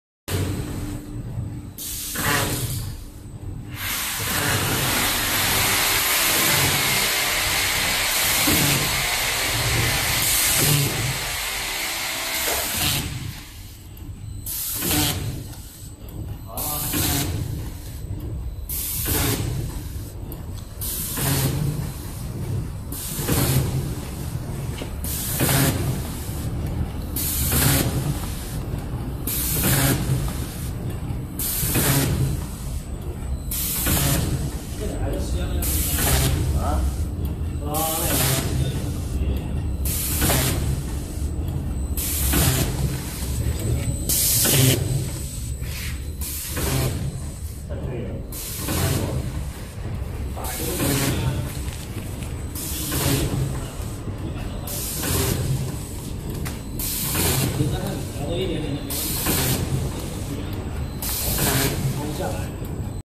Automatic soluable PE film powder packing machine with volumetric cup metering
Technical parameters

model

AZ-GP100

Power/voltage

1800W/220v or 110V

Bag size

L: 40-160 (mm) W: 20-140 (mm)

Measuring range 10-120G/other specifications can be customized

Sealing form Three side seal, back seal

Scope of application: suitable for wolfberry, hardware accessories, tea, etc.